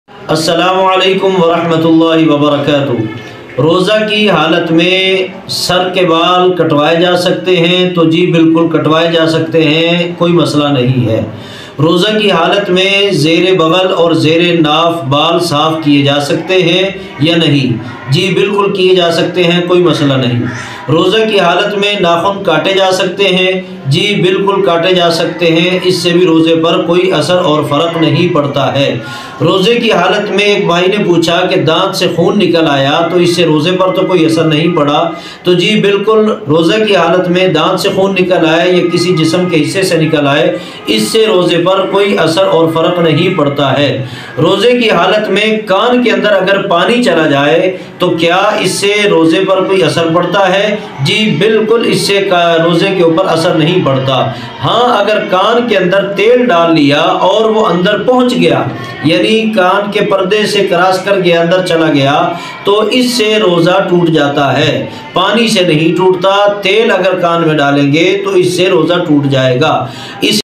Roza me Baal aur Nakhun katna Kaisa hai? Roza me kan me tel dalna |Molana Umar Farooq Muhammadi
Roza k Masail
Roza ki halat me Nakhun katna Kaisa hai
Roza ki halat me khon Nikalna
Ramzan corse
Molana Umar Farooq Muhammadi